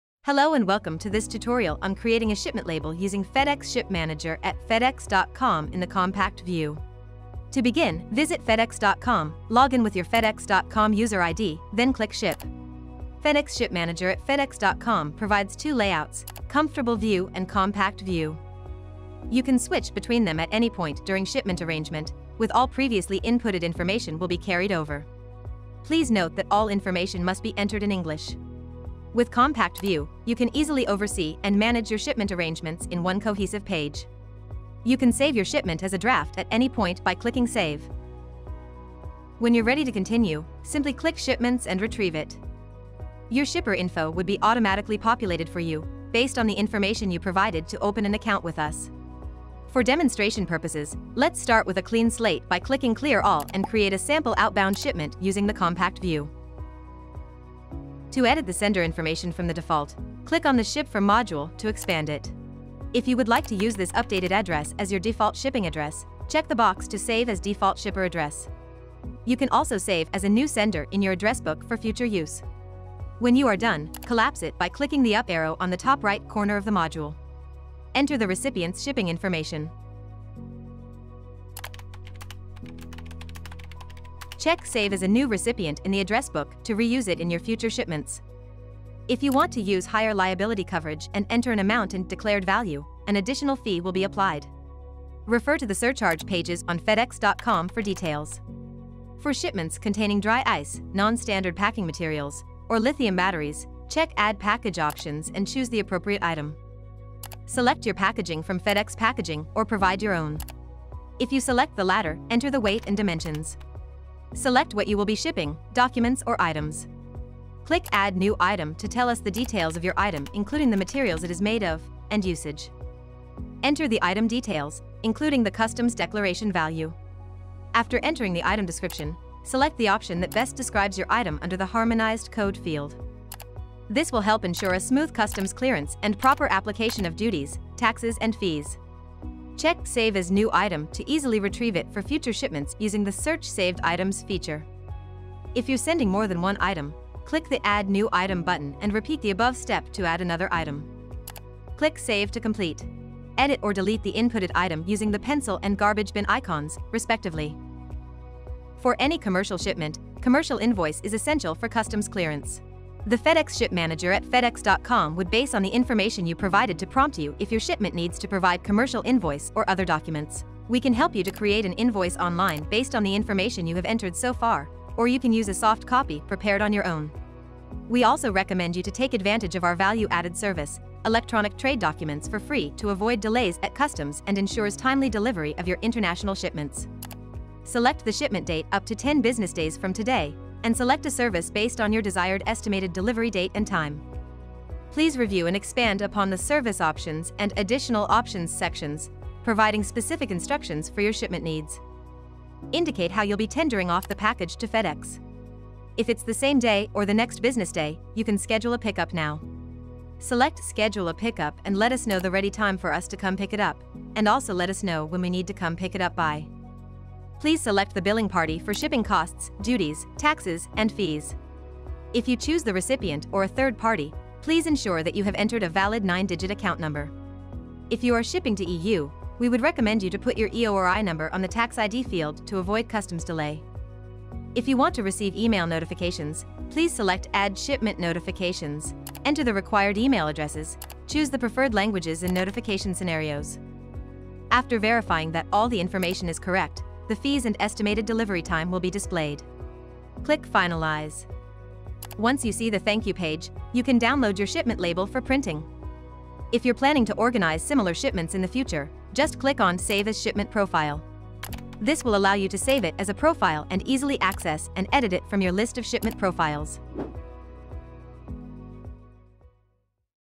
Creating a shipment label using FedEx Ship Manager™ at fedex.com in the Compact View
00:00 - 00:12 Introduction
00:12 - 00:54 Compact View and basics
00:54 - 01:29 Sender and recipient
01:29 - 02:27 Package details and trade documents
02:27 - 03:21 Service and Additional options
03:21 - 03:37 Pickup/drop off
03:37 - 03:54 Billing and Tax ID
03:54 - 04:03 Add shipment notifications
04:03 - 04:26 Finalize and save shipment as a profile